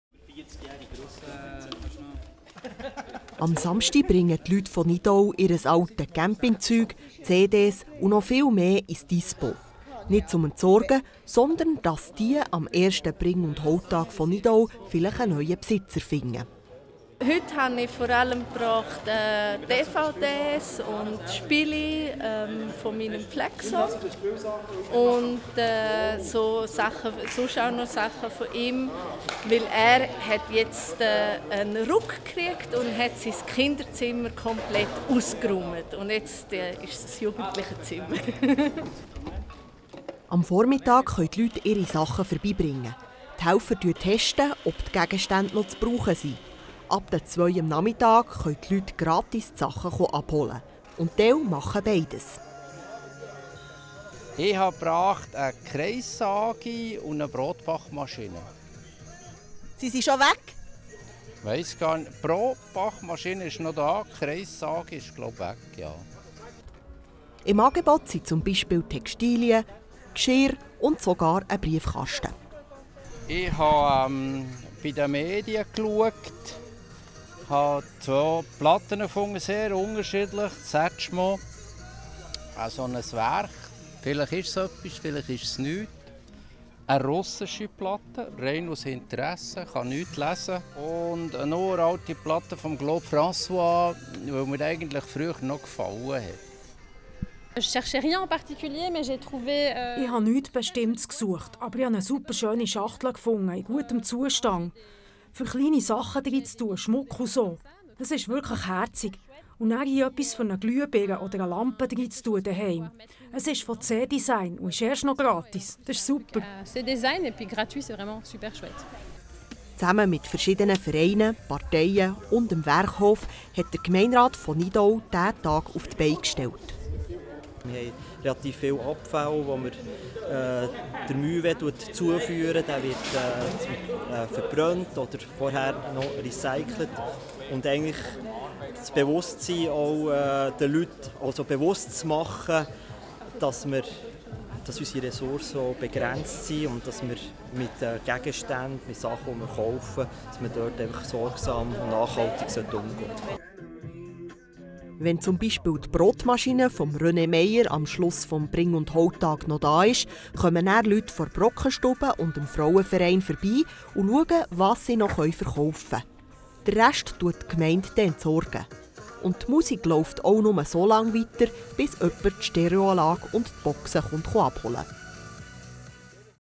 INFO : Erster Bring- und Holtag in Nidau / TeleBielingue
Erster Bring- und Holtag in Nidau
Gratis für die Bevölkerung von Nidau: Haushaltsgeräte, Kleider, CDs usw. - alles gebracht von Mitbürgerinnen und Mitbürgern.

Le premier "Bring-und Holtag" à Nidau
Appareils électroménagers, habits, CDs : les Nidowiens ont pu se débarrasser d'objets ou en choisir d'autres gratuitement samedi.